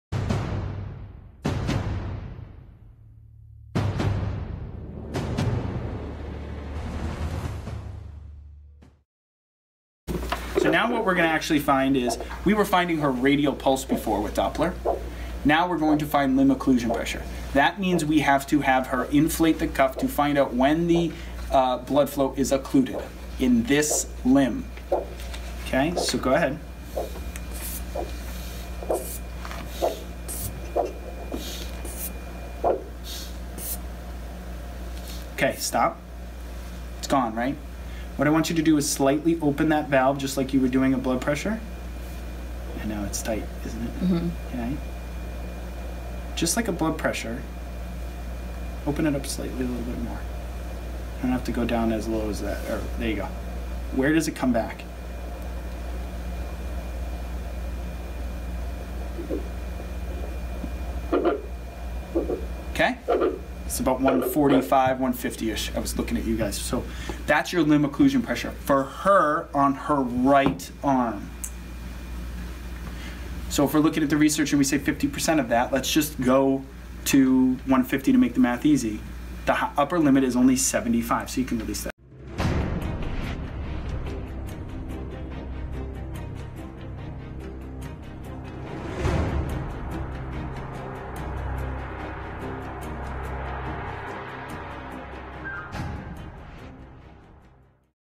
Finding Limb Occlusion Pressure LOP for BFR
This video describes how simple it is to find limb occlusion pressure (LOP) to use when individualizing pressures for blood flow restriction therapy/training. LOP can change based on a variety of factors so it is important to get this information every time you use BFR with a clinical patient.